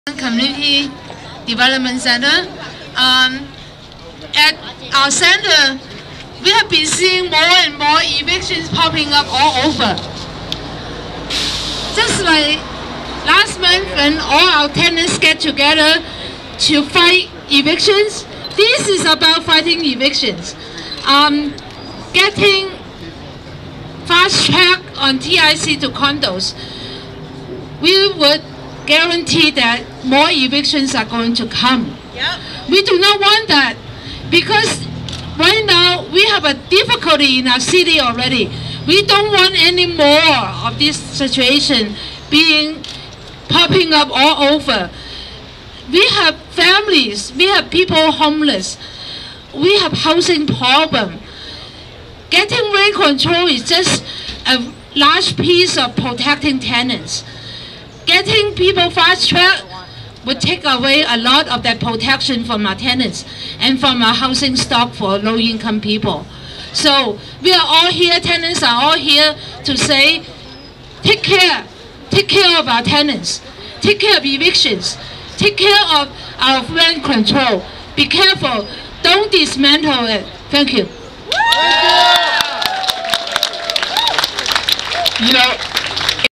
Part IX of January 28, 2013, Protest Proposed TIC Law Lifting Limit on SF Condominium Conversions
Part IX of January 28, 2013, Protest of Proposed TIC Law Lifting Limit on San Francisco Condominium Conversions, Threatening Rent Control (VID00009.MOV)